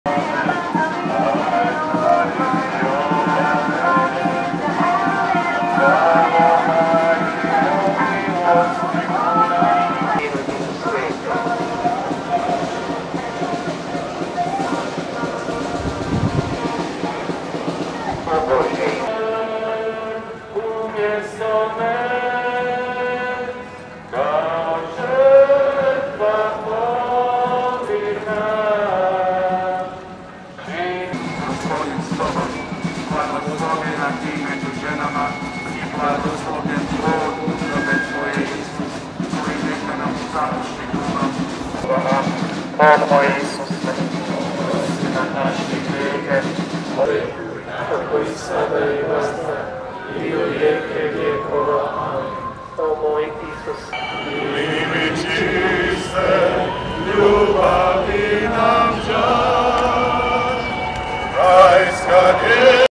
Muškarci na molitvi krunice na Trgu bana Jelačića
VIDEO: Lovro Domitrović/PIXSELL